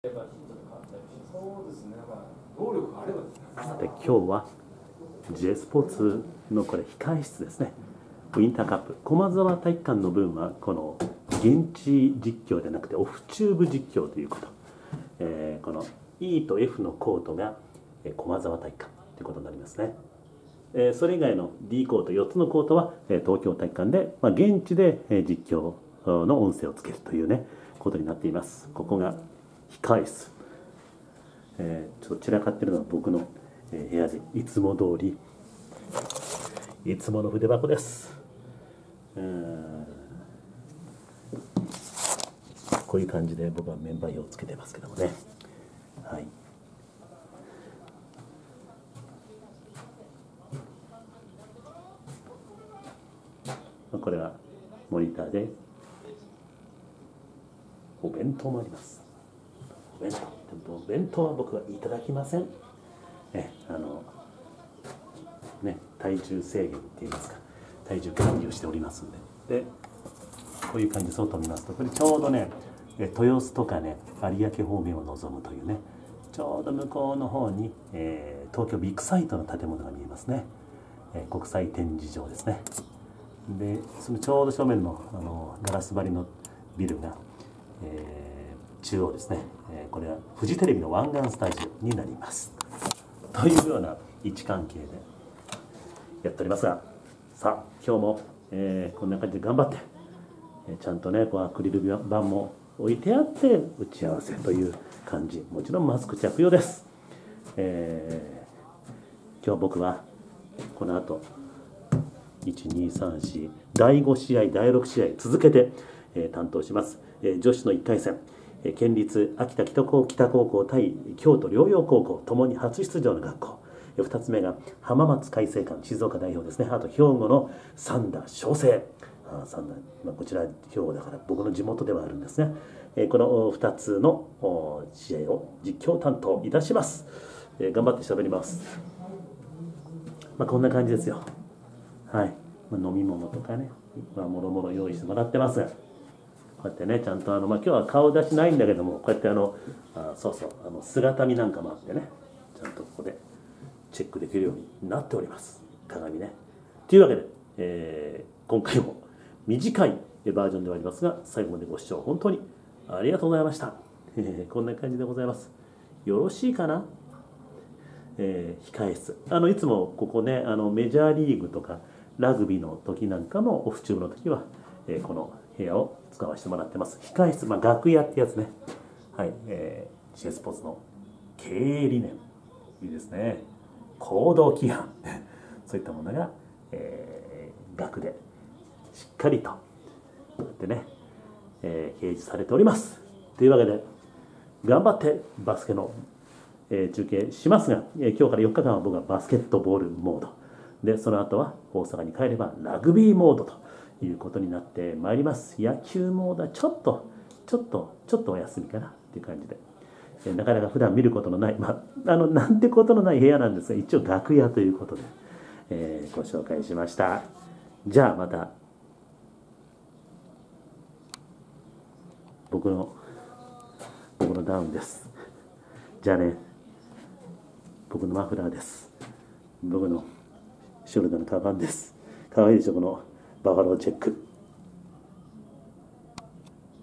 KazuTube!今日は東京・青海のJSPORTSさんにお邪魔しておりまして・・・・バスケのウインターカップの実況です！これが、楽屋ですわ！
駒沢体育館の試合はスタジオからのオフチューブです！